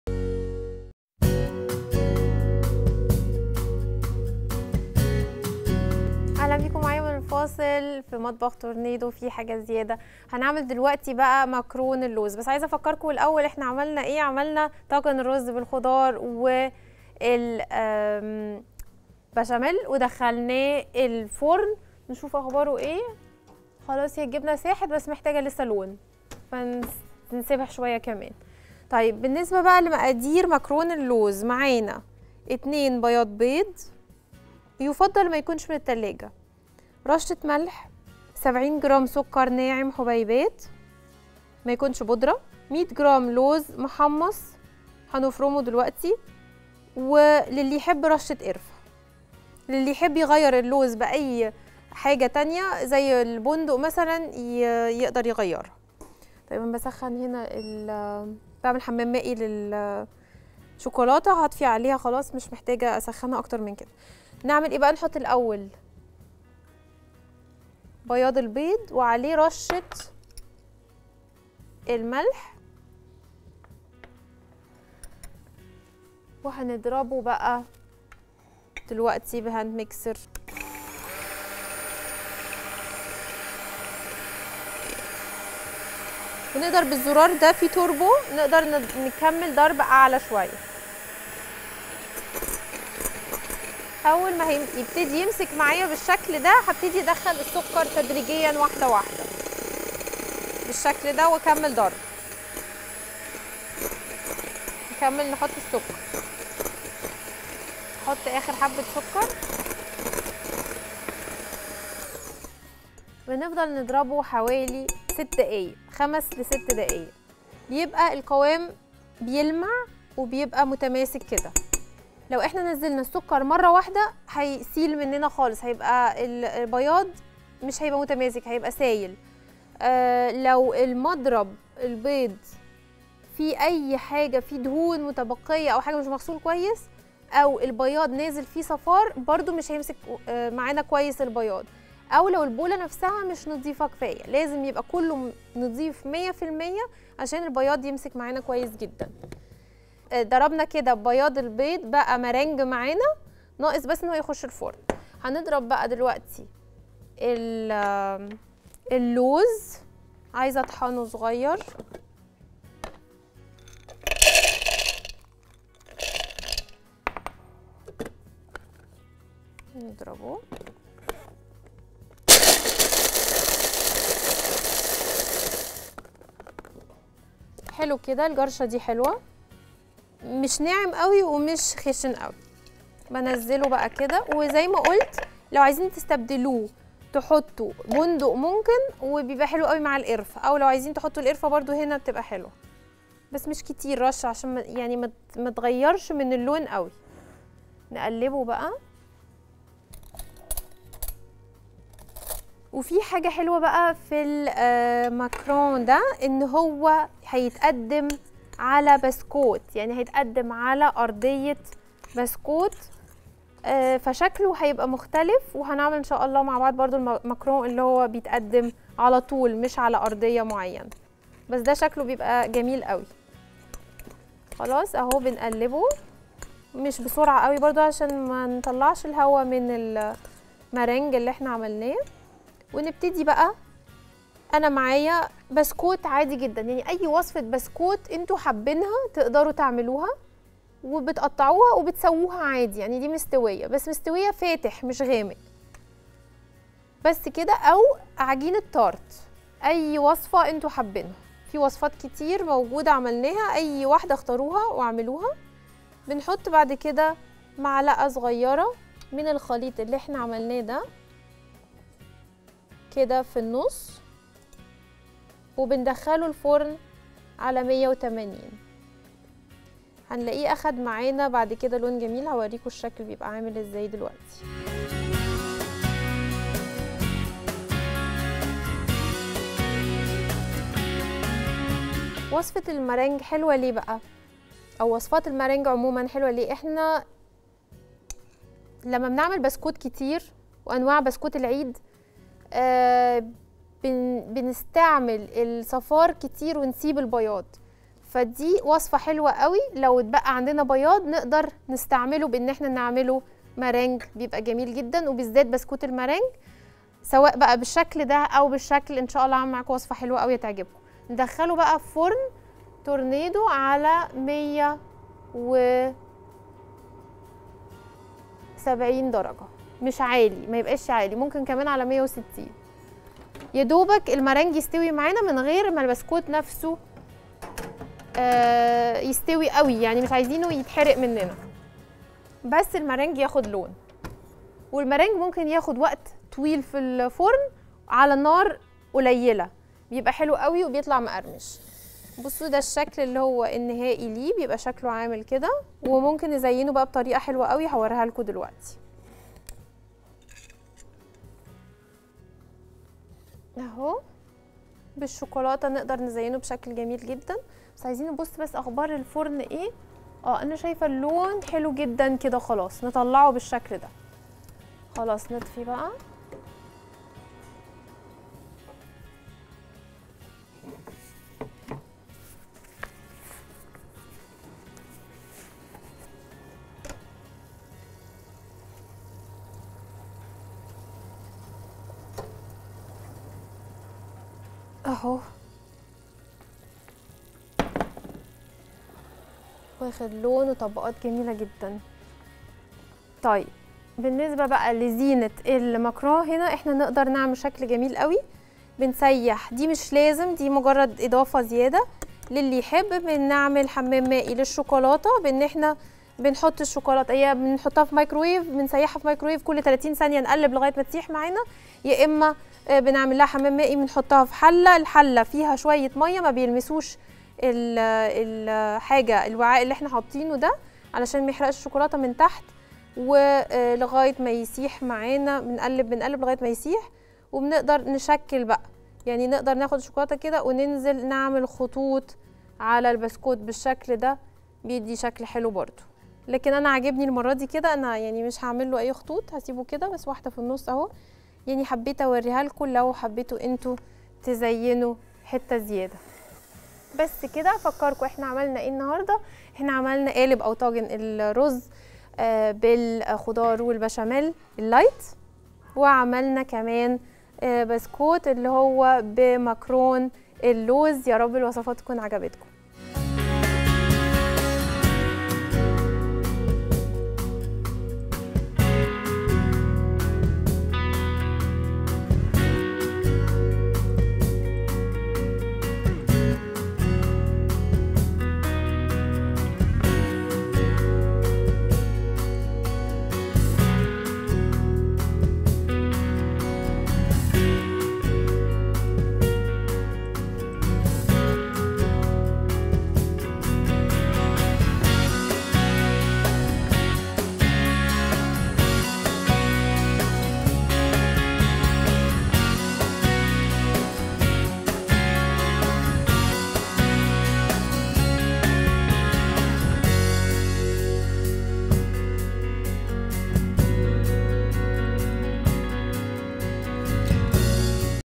ماكرونز باللوز | سالي فؤاد
طريقة عمل ماكرونز باللوز مع سالي فؤاد في مطبخ تورنيدو